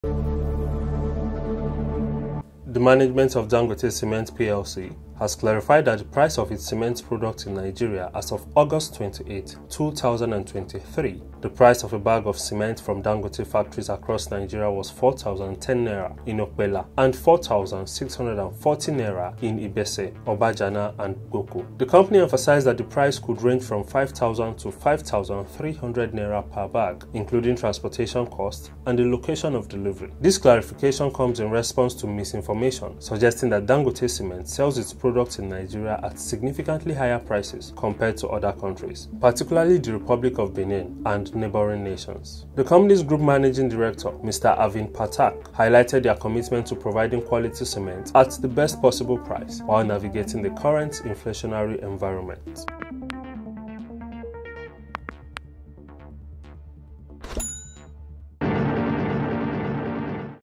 Dangote Cement clarifies misinformation about price of product
The management of Dangote Cement Plc has clarified the price of its cement products in Nigeria. As of August 28, 2023, the price of a bag of cement from Dangote factories across Nigeria was N4,010 in Okpella and N4,640 in Ibese, Objana, and Gboko.

The company emphasized that the price could range from N5,000 to N5,300 per bag, including transportation costs and the location of delivery. This clarification comes in response to misinformation suggesting that Dangote Cement sells its products in Nigeria at significantly higher prices compared to other countries, particularly the Republic of Benin and neighbouring nations.

The company's Group Managing Director, Mr Arvind Pathak, highlighted their commitment to providing quality cement at the best possible price while navigating the current inflationary environment.